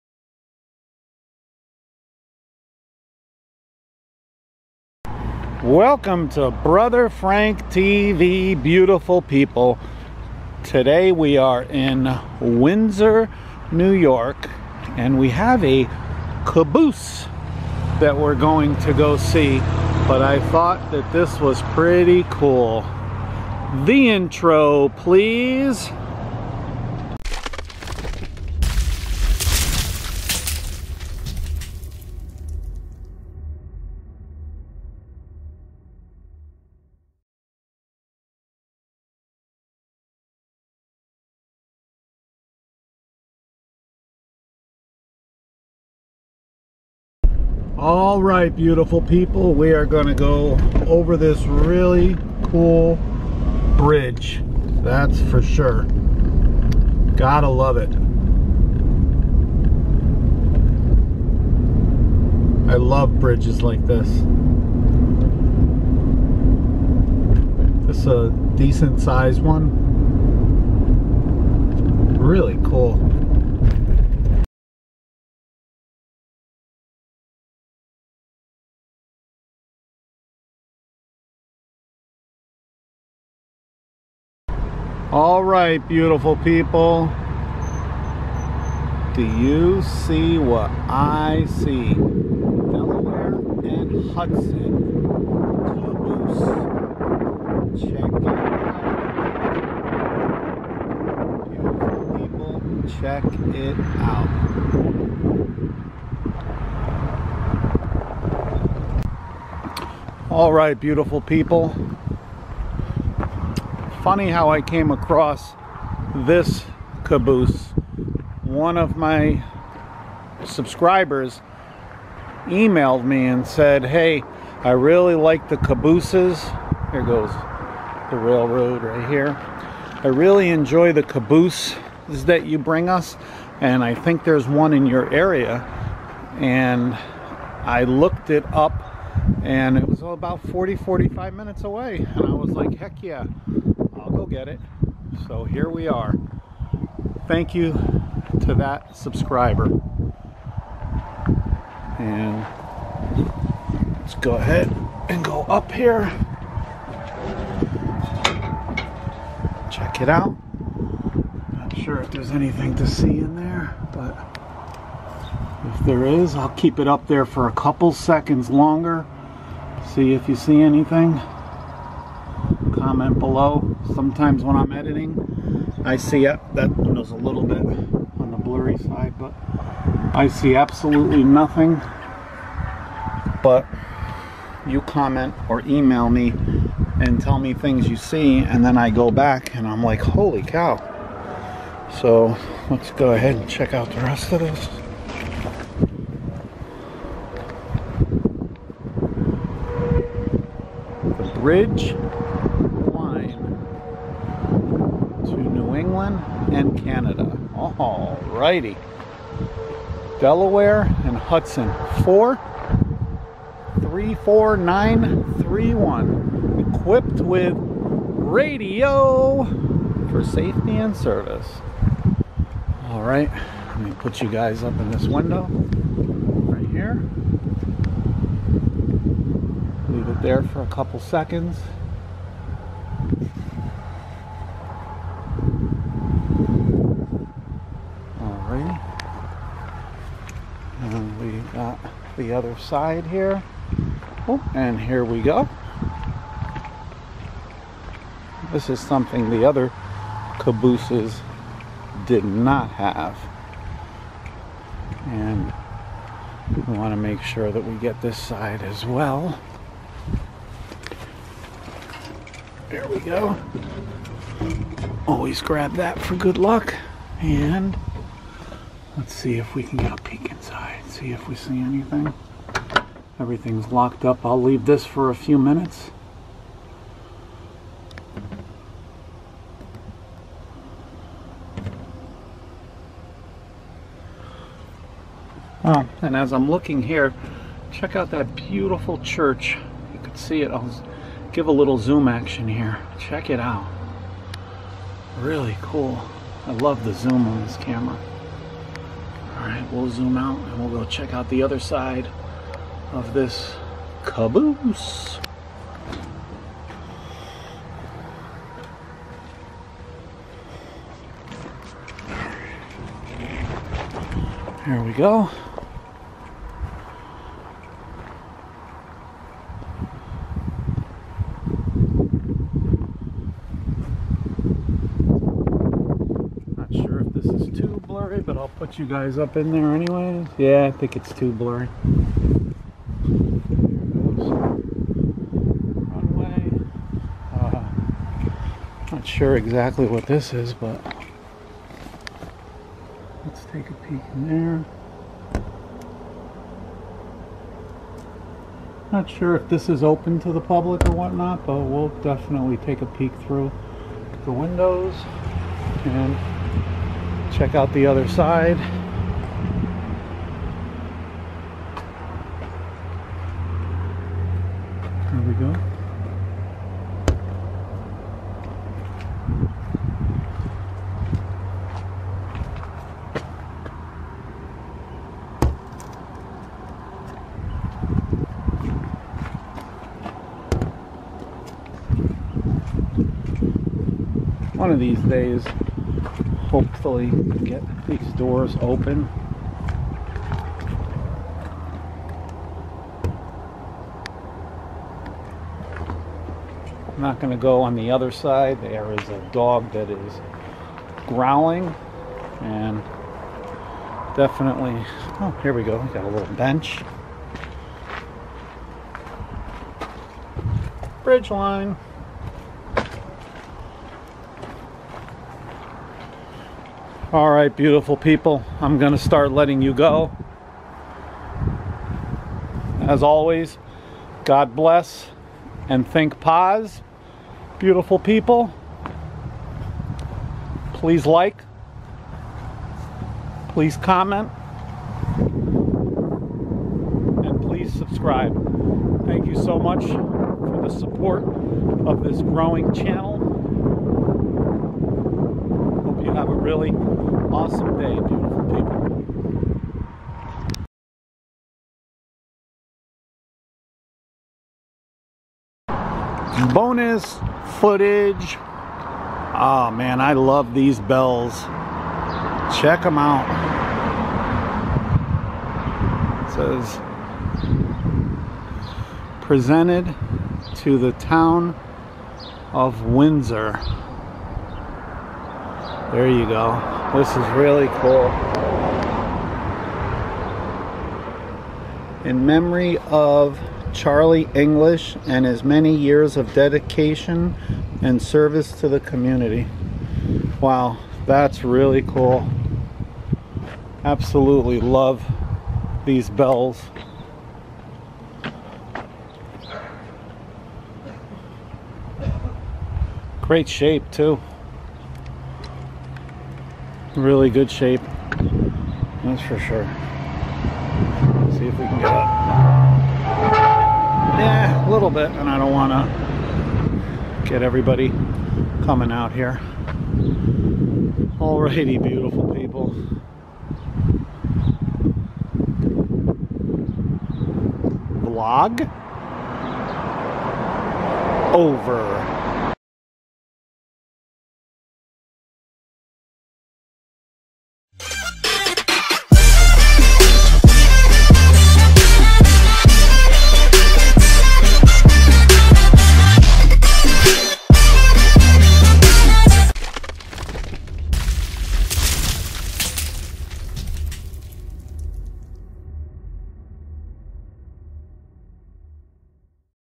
"Train Caboose" at Windsor Train Station!!!
The Windsor train station is the sole remaining structure on the former "mainline" of the Delaware and Hudson Railroad which connected Lanesboro, Pennsylvania to Nineveh Junction, New York... The Delaware and Hudson Railroad started in Carbondale, Pennsylvania, and is the only line to have gone through Windsor.  The Susquehanna Valley was one of the last large areas of the state to receive the benefit of railroad access. I also found a very cool "Bell" in front of town hall. A Must Visit If In Area Beautiful Peeps!

God Bless & Think POZ!